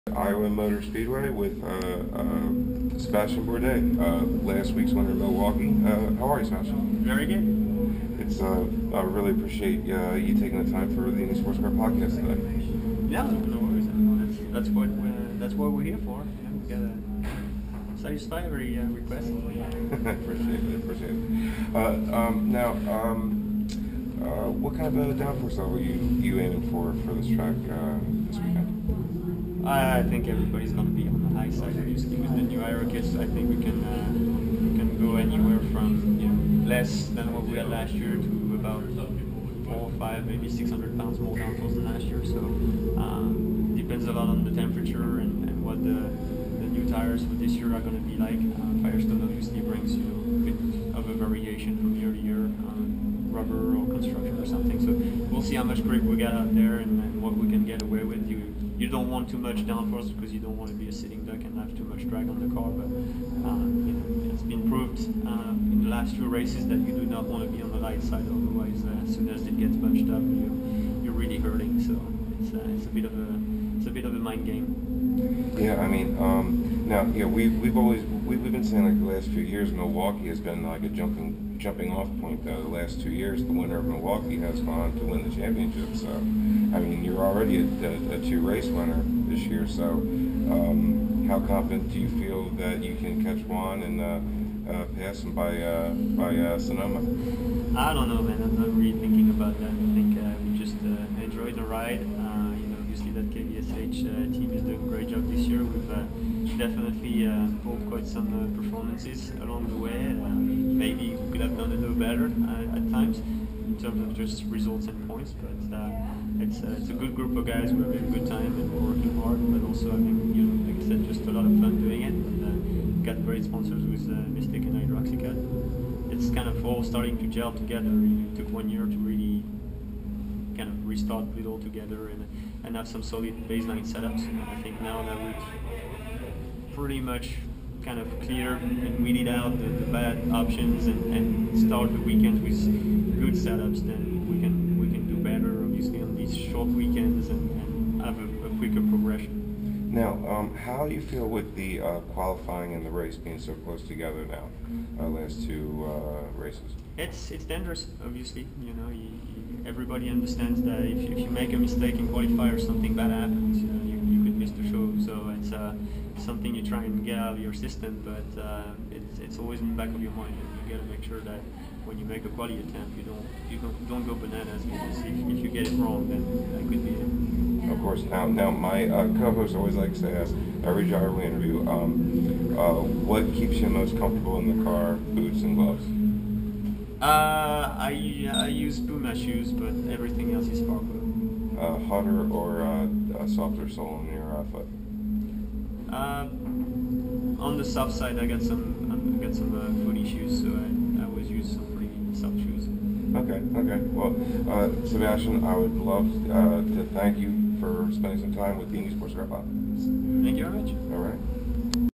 Sebastien Bourdais Interview Iowa 2015
@LlNKZ interviews Seabass for the Indy Sportscar Podcast at iowa speedway 2015